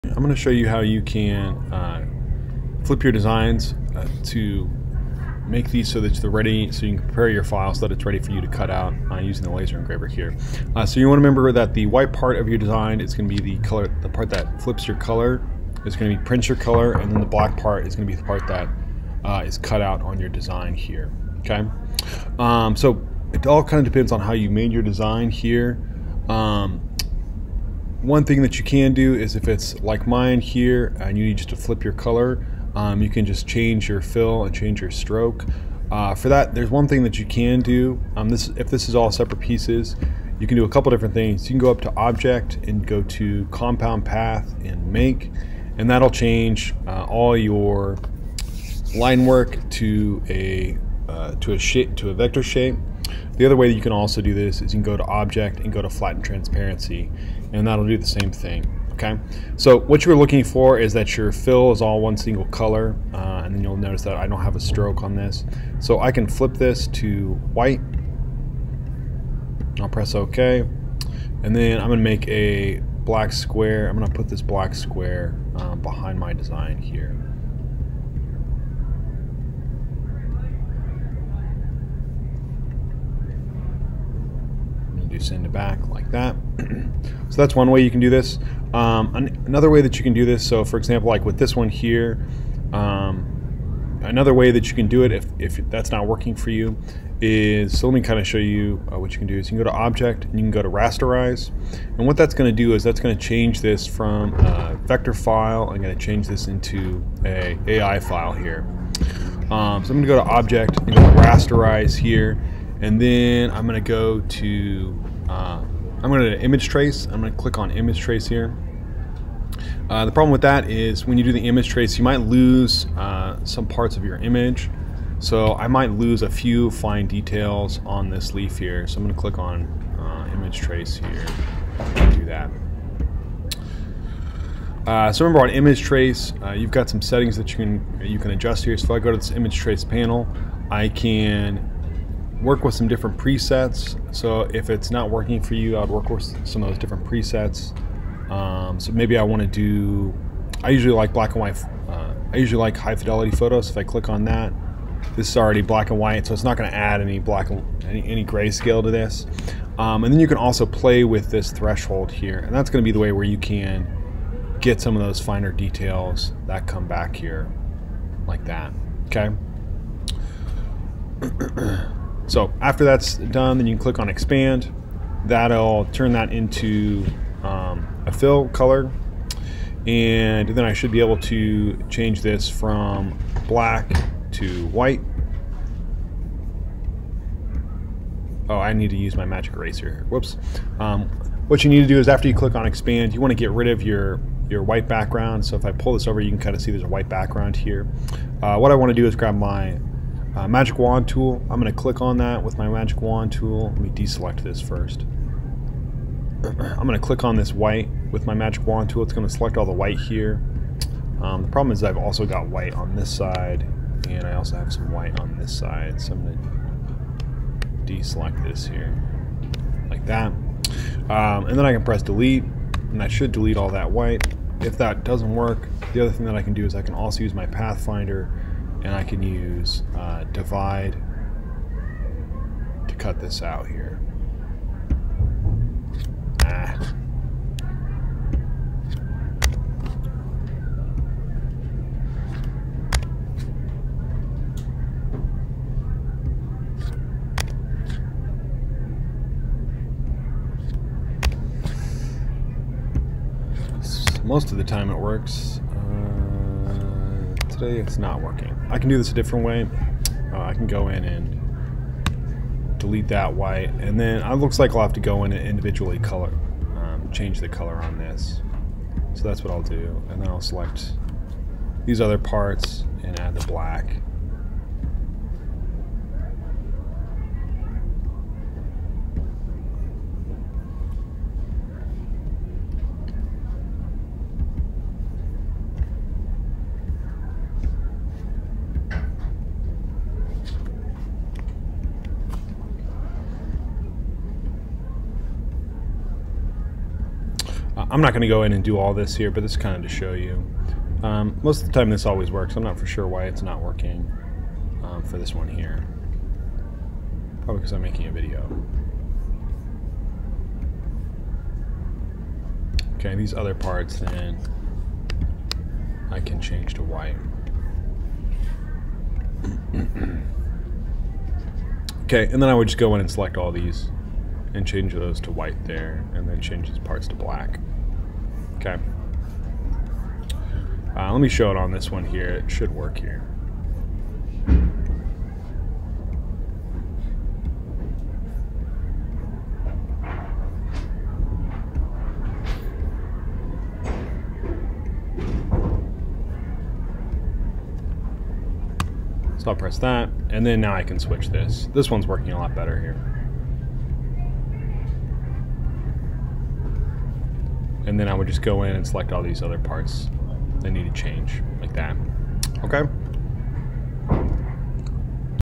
Kamon Crest: Rasterize
How to prepare and rasterize your file for you Kamon Crests